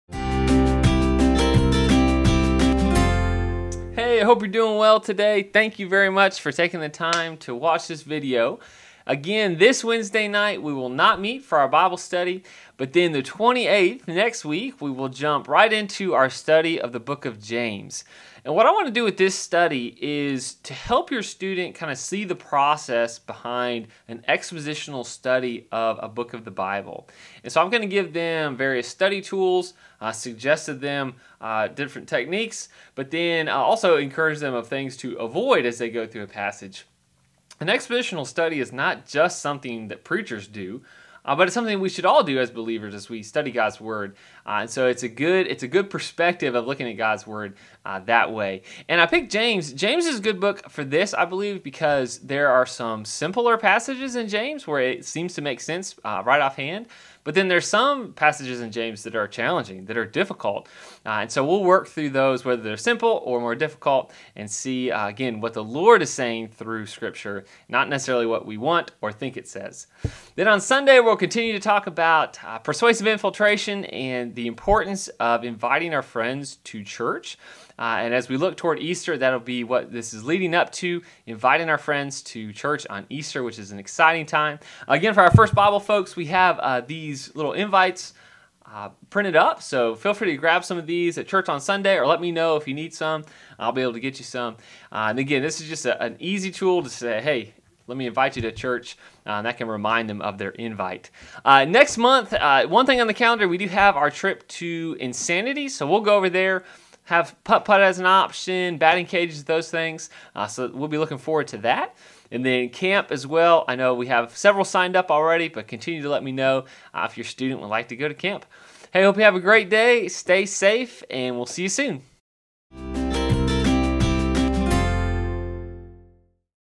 03.19.18
This is Parent Video #63!

No WNBS this week!
James Study starts next week!

Sunday AM: Persuasive Infiltration (pt.2)

Insanity next month and Camp registration is open!